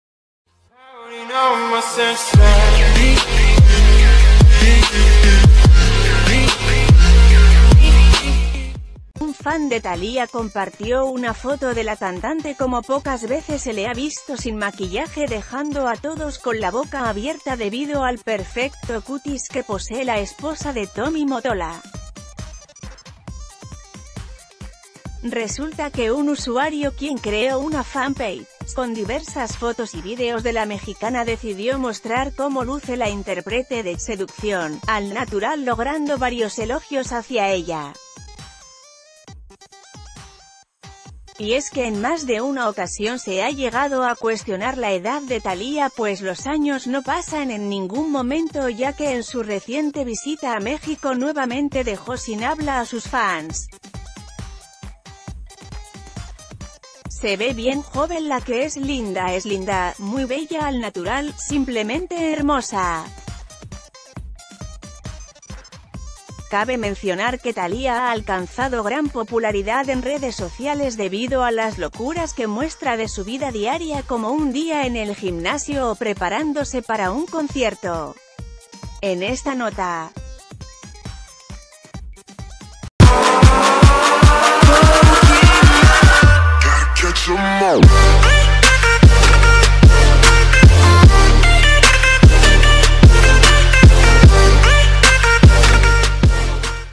Thalía impacta a sus millones de fans sin nada de maquillaje
Un fan de Thalía compartió una foto de la cantante como pocas veces se le ha visto sin maquillaje dejando a todos con la boca abierta debido al perfecto cutis que posee la esposa de Tommy Mottola. Resulta que un usuario quien creo una fan page con diversas fotos y videos de la mexicana decidió mostr...